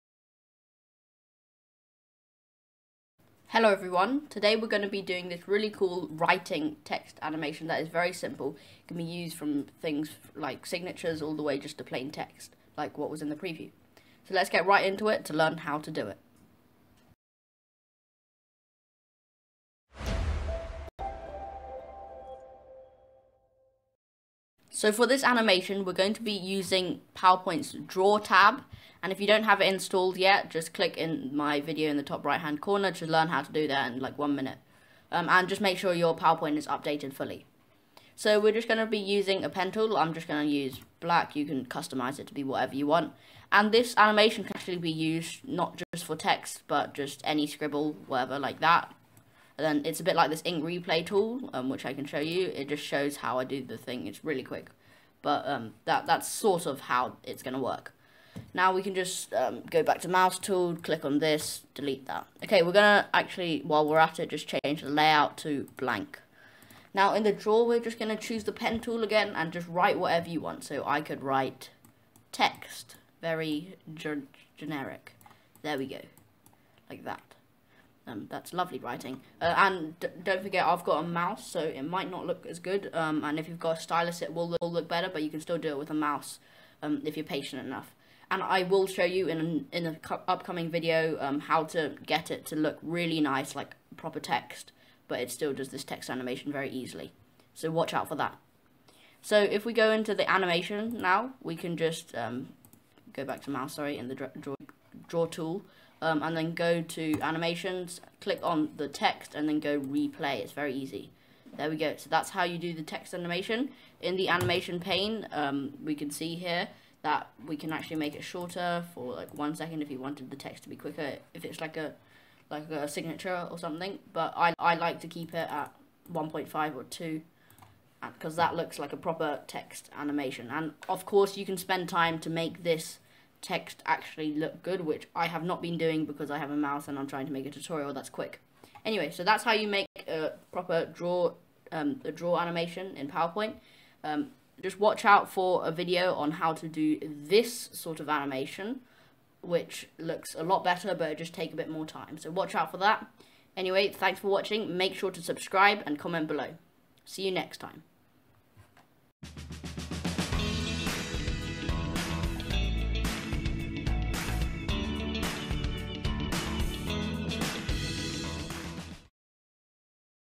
EASIEST Writing Text Animation in PowerPoint | Tutorial
Learn this really easy writing animation in PowerPoint, under 3 minutes.

EASY Writing Text Animation in PowerPoint | Tutorial

🔸 PowerPoint Tutorials
🔹 PowerPoint Animation
🔹 How to make Professional Infographics